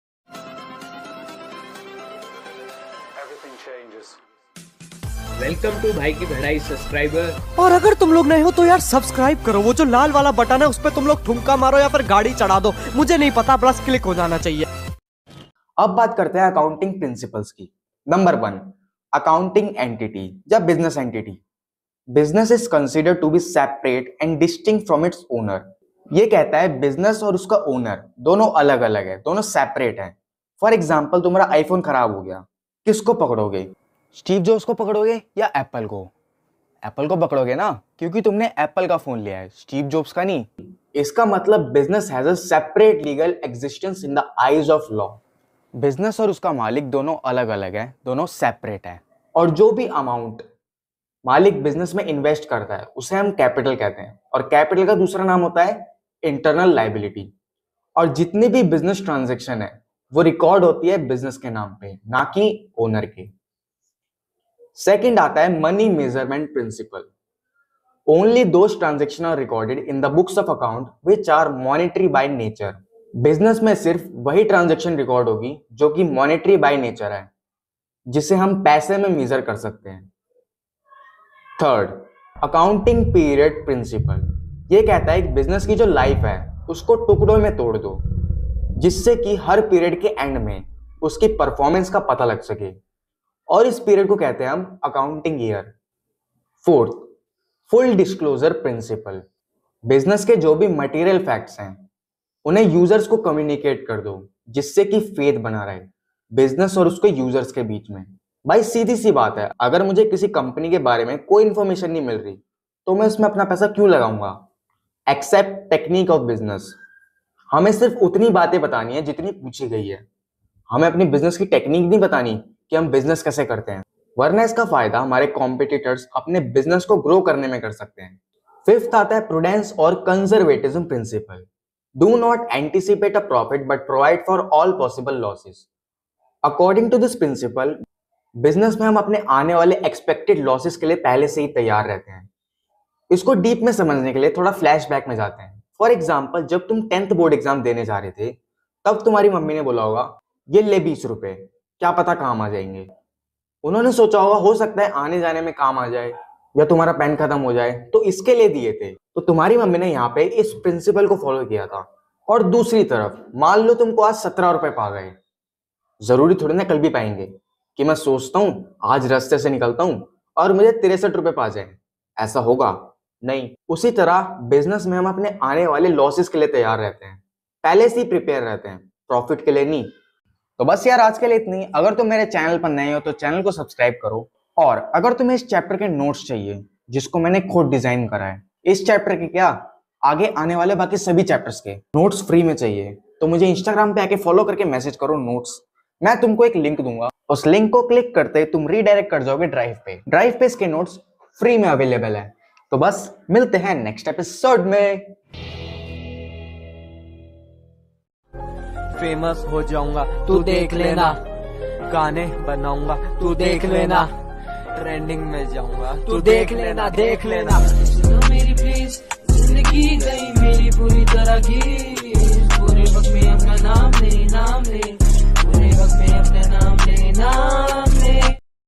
Accounting Principles | ep-02 | Class 11 | Accounts | Chapter 3
0:00 @BhaiKiPadhai [Credit]
0:15 (1) Business Entity
1:04 (2) Money Measurement Principle
1:20 (3) Accounting Period Principle
1:35 (4) Full Disclosure Principle
2:08 (5) Conservatism Principle
3:11 PDF Notes
3:24 to be continue